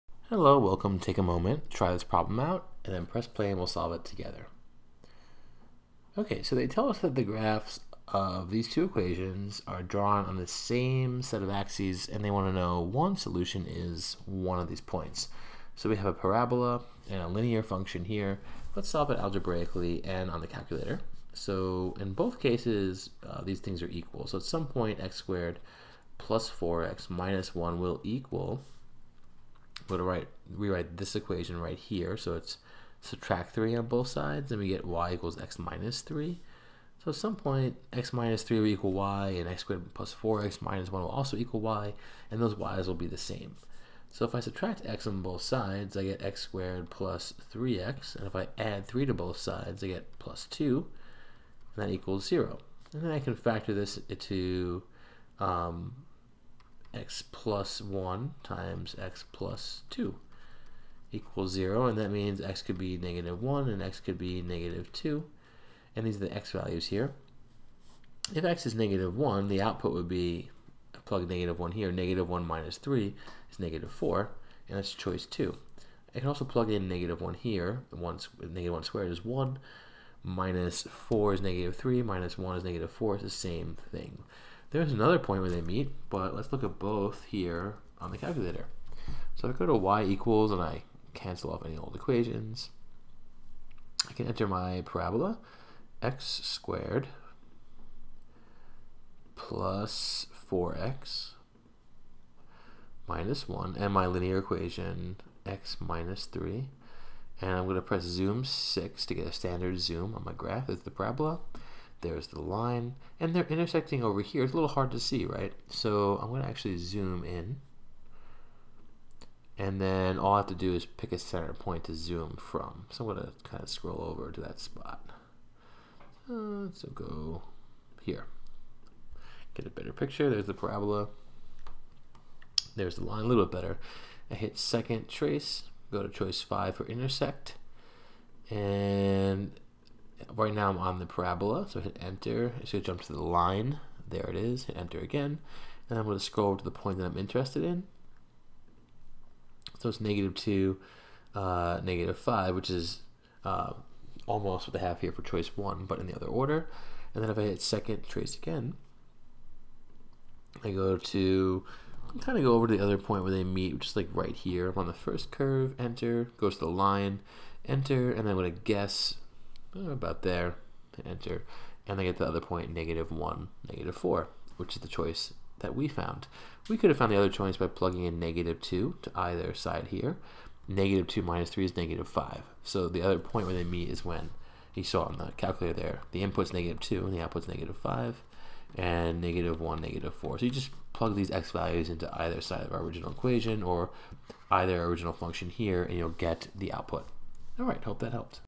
Algebra 2 Regents June 2018 #01
In this video, we work through a probability example from the June 2018 Algebra 2 Regents exam.